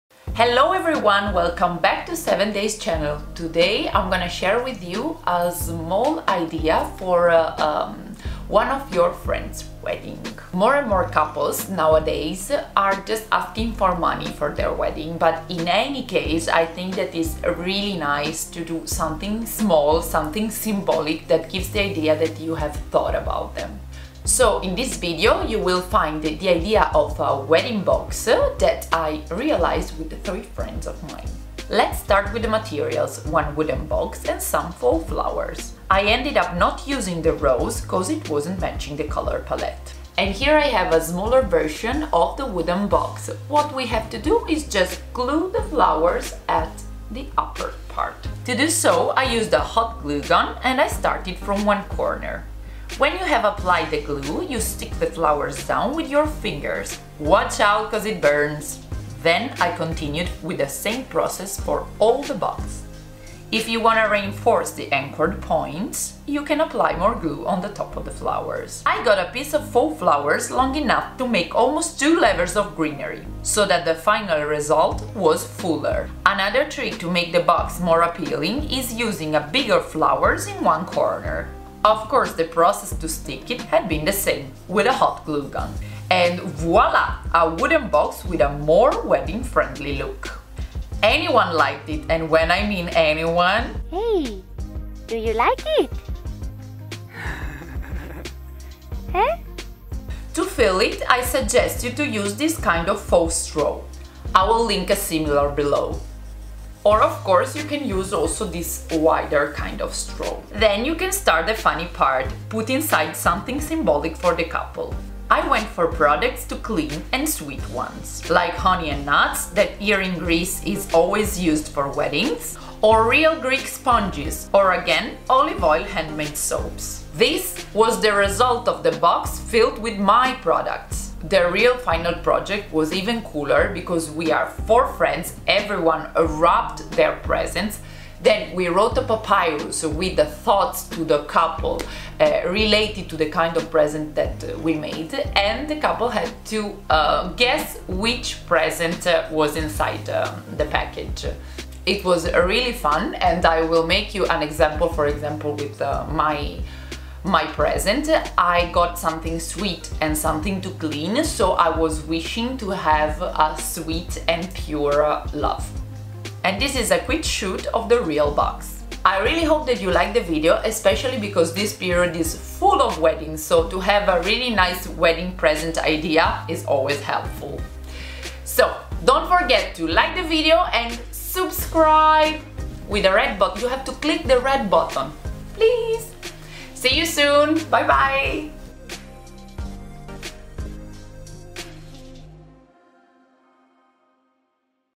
WEDDING BOX IDEA
This is an idea for an extra symbolic present for the couple.
You can prepare a box of goodies that represent their love. es. If you put a soap you can wish a pure love, or if you put seeds, you can wish to their love to grow.
And it is really important to spice up a bit the box: with flowers, with bows, with pearls.

Audio from youtube library